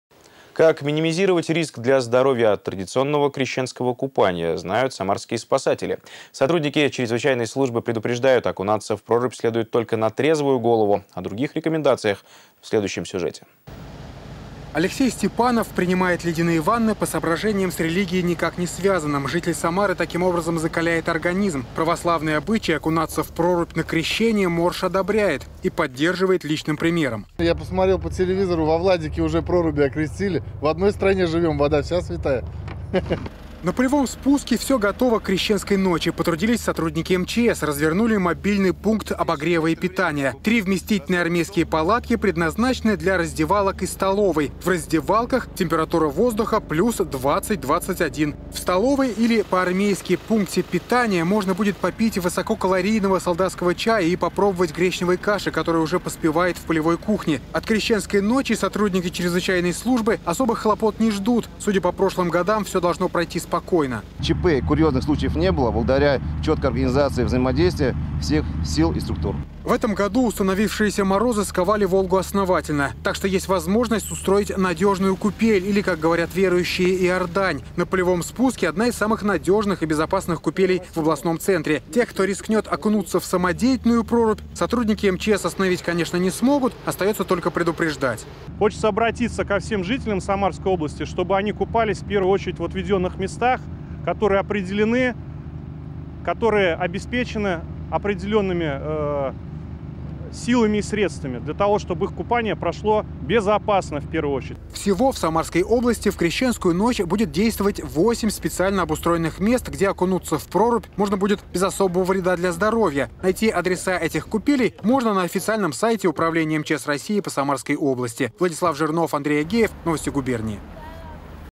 Как в Самаре подготовились к Крещению?
Как минимизировать риск для здоровья от традиционного крещенского купания знают самарские спасатели. Сотрудники чрезвычайной службы предупреждают - окунаться в прорубь следует только на трезвую голову. О других рекомендациях подробнее в нашем сюжете.